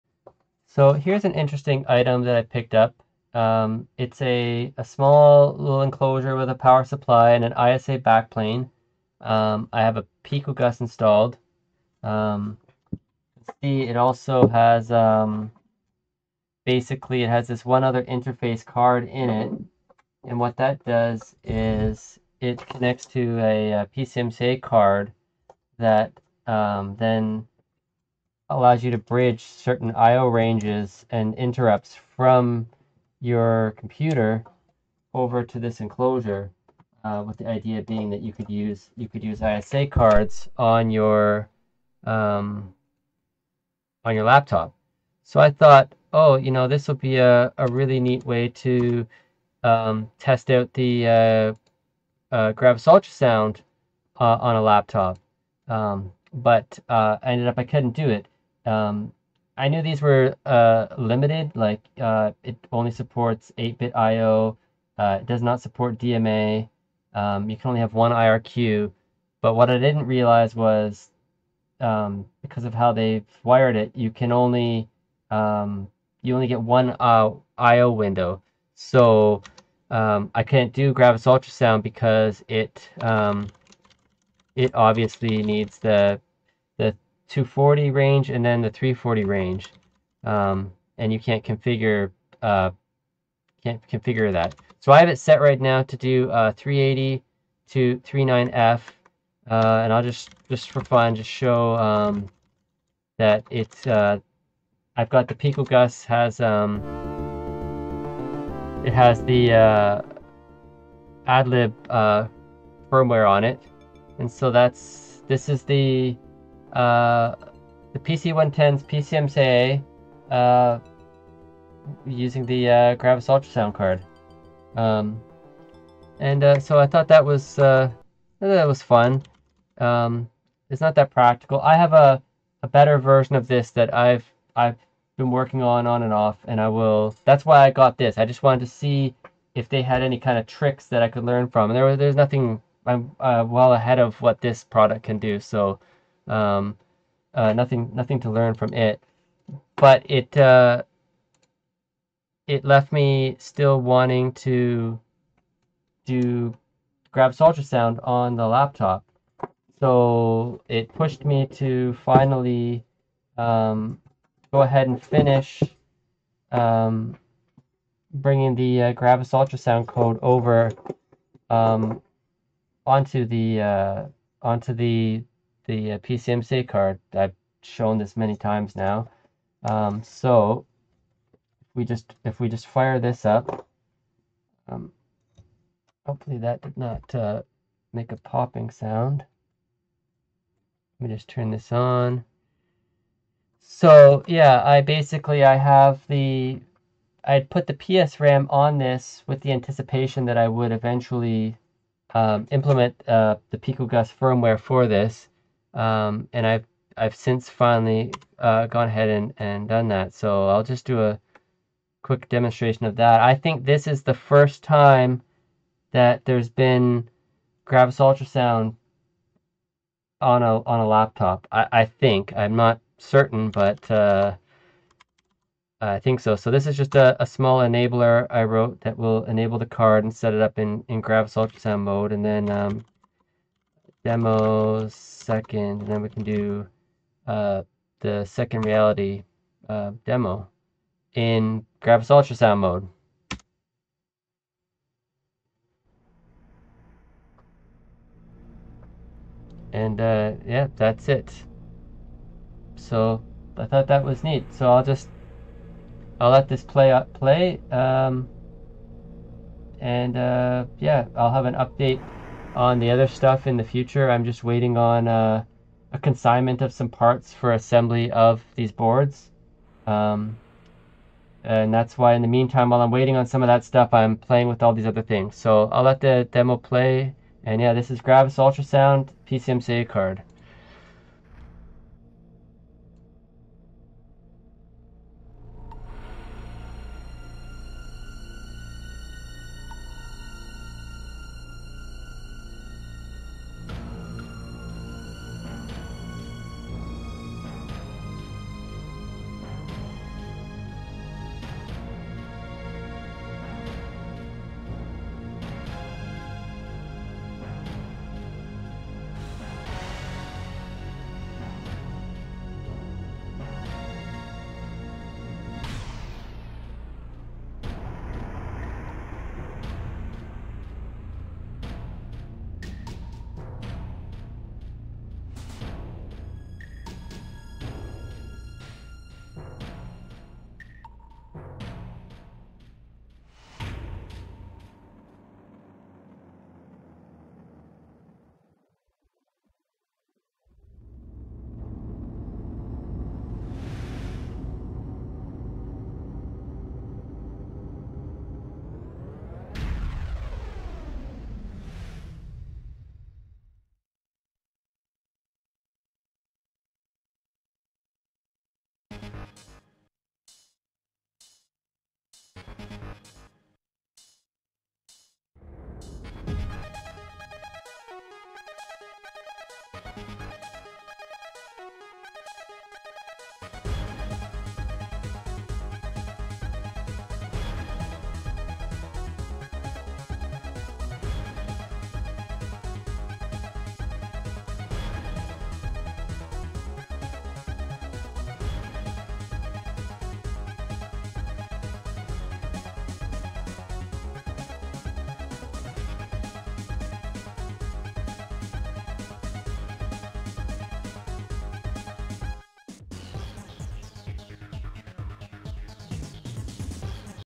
PCMCIA Gravis Ultrasound (PicoGUS) & ISA Expander
Quick demo of the ISA PicoGUS being used via a PCMCIA expander,  and of the PicoGUS Gravis Ultrasound emulation running on the PicoPCMCIA card.

If you are interested in getting one of these cards when it released, please fill out the following questionare

0:00 ISA Expander
1:45 PicoGUS Adlib via ISA Expander
3:45 Gravis Ultrasound Demonstration